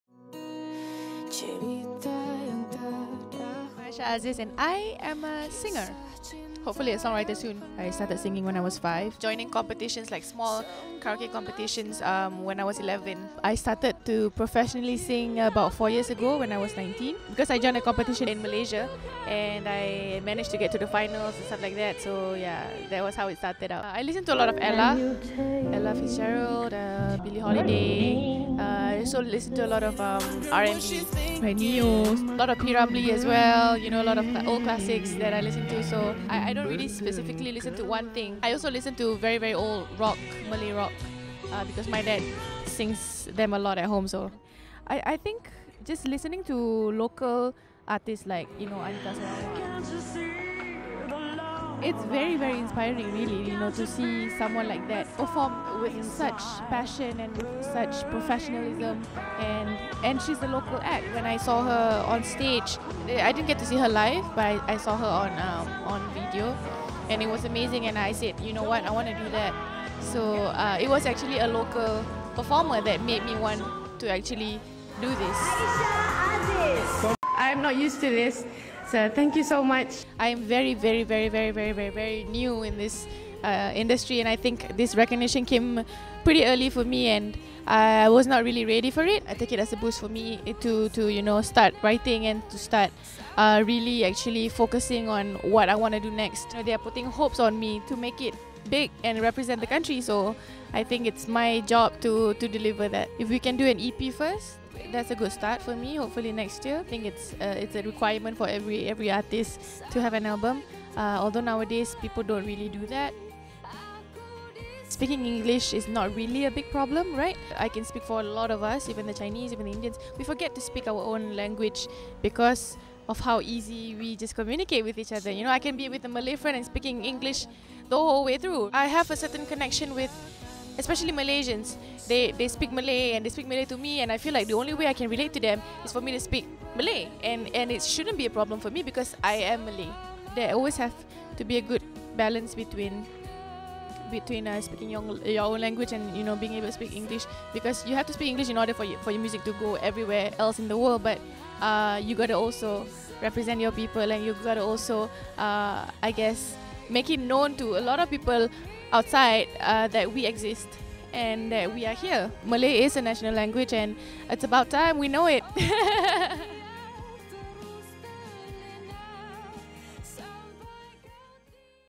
A conversation with Aisyah Aziz about her inspirations, representation in music and her future plans
Last year Bandwagon had a conversation with Singaporean singer-songwriter Aisyah Aziz about how she got her start in music, being inspired by Anita Sarawak and the importance of singing in your mother tongue or first language.

Filmed by: Chong Yoke Ming
Edited by: Hazel Sim